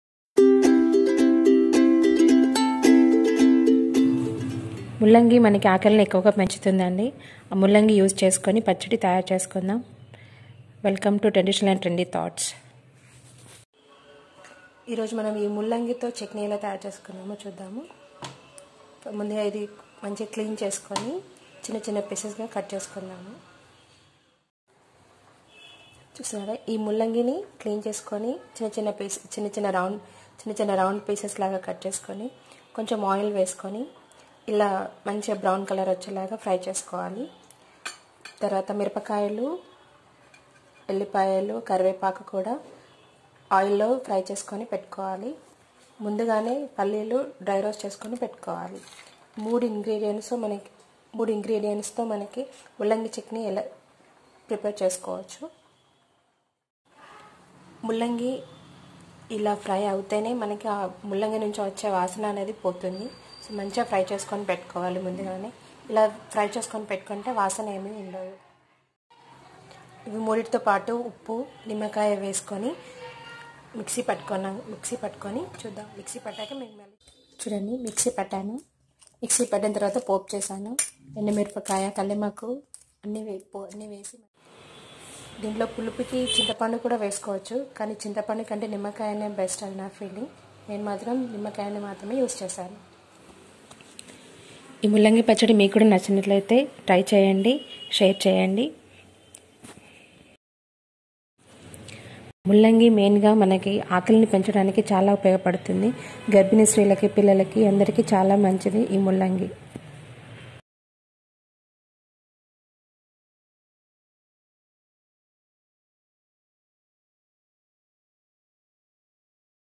mullangi pachadi healthy and tasty.. 👌👌👌
Mullangi
Mirchi
Garlic
Lemon
Salt
Peanuts
Try this simple recipe and keep watching my videos.. Please hit bell icon to get my ne vudeos..Thankyou..👍👍👌